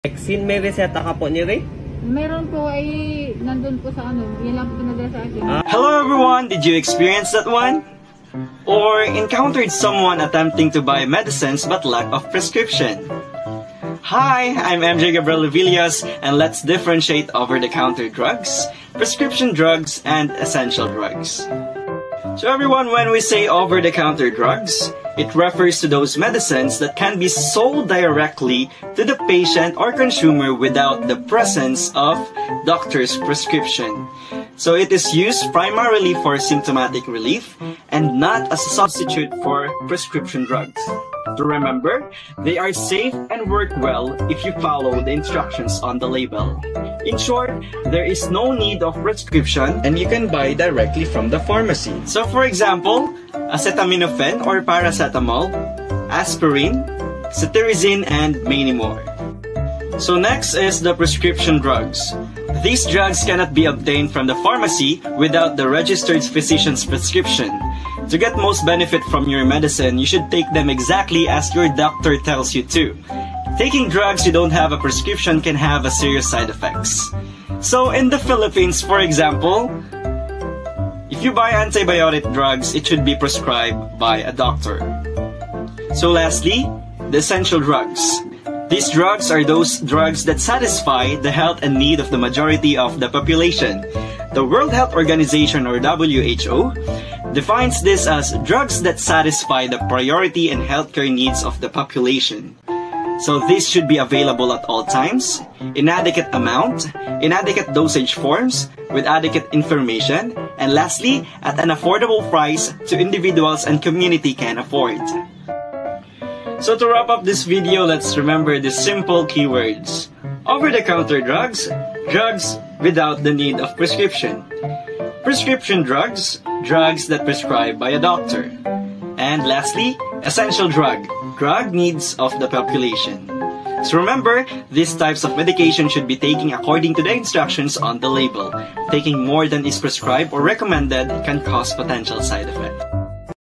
VET PHARMA 1 | OTC vs Prescription vs Essential drugs | Chapter 1 (2022)
This is a student output presentation (Group 1A) about "OTC vs Prescription vs Essential drugs" and this is presented by 3DVM-A students of Basic Veterinary Pharmacology for the academic year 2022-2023.